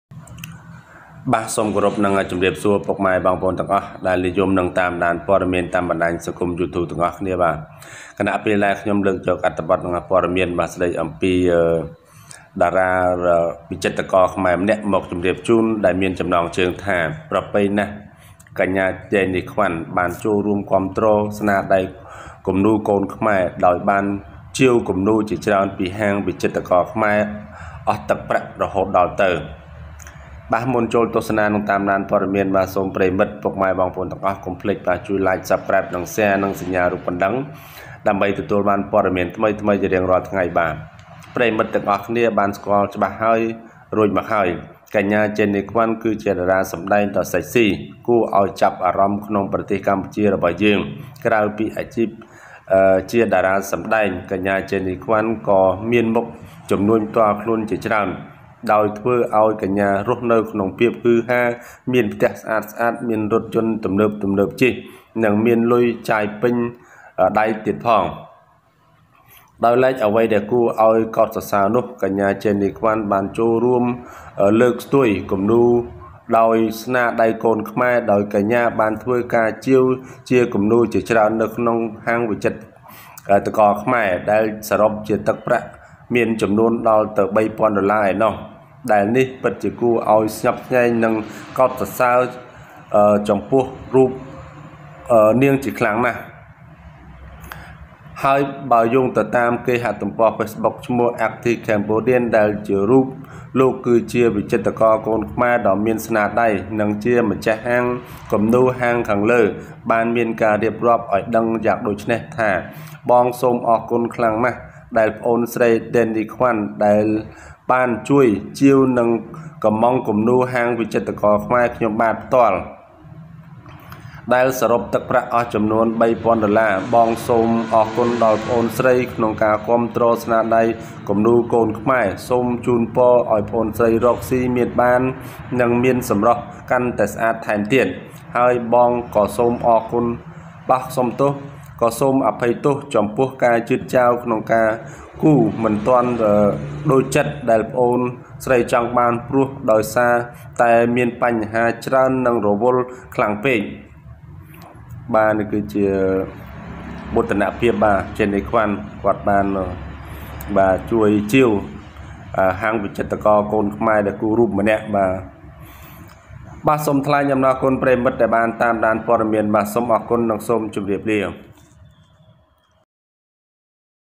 ប្រពៃណី​ណាស់​ កញ្ញា​ ដេនី​ ក្វាន់​ បានចូលរួមគាំទ្រស្នាដៃគំនូរកូនខ្មែរ​ ដោយបាន​ជាវ​គំនូរ​ជាច្រើន​ពីហា
ប្រពៃណី​ណាស់​ កញ្ញា​ ដេនី​ ក្វាន់​ បានចូលរួមគាំទ្រស្នាដៃគំនូរកូនខ្មែរ​ ដោយបាន​ជាវ​គំនូរ​ជាច្រើន​ពីហាង​ វិចិត្រ​ករ​ខ្មែរ​ អស់ទឹកប្រាក់​រហូត​ដល់ទៅ​​